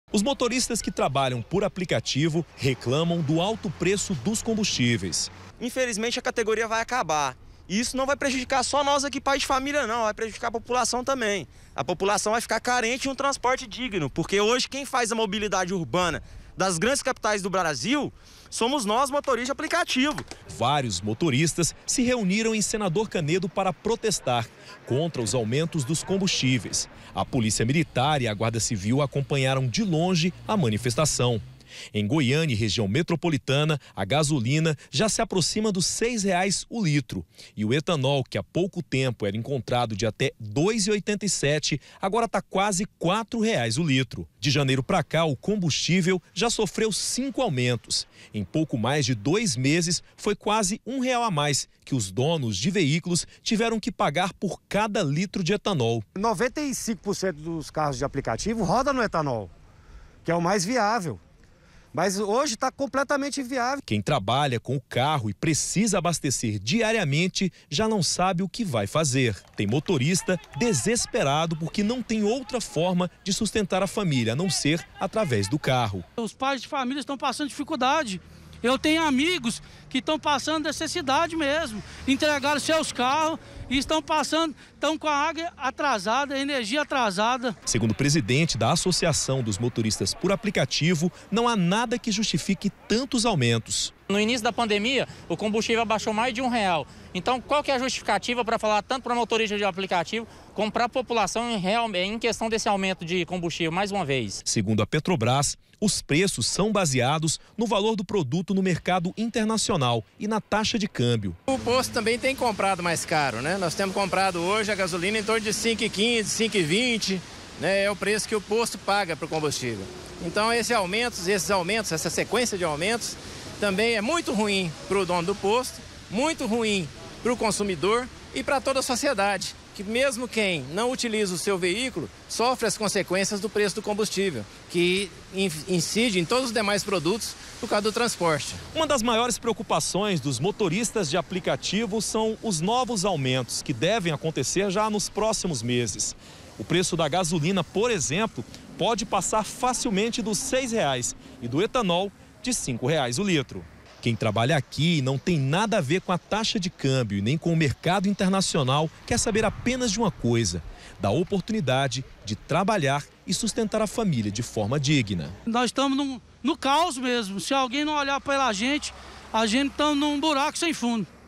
COMBUSTÍVEIS: MOTORISTAS PROTESTAM CONTRA MAIS UM AUMENTO
MOTORISTAS DE APLICATIVO FIZERAM HOJE UM PROTESTO CONTRA O AUMENTO DOS COMBUSTÍVEIS. A MANIFESTAÇÃO ACONTECEU EM SENADOR CANEDO, ONDE FICA UMA DISTRIBUIDORA DA PETROBRAS.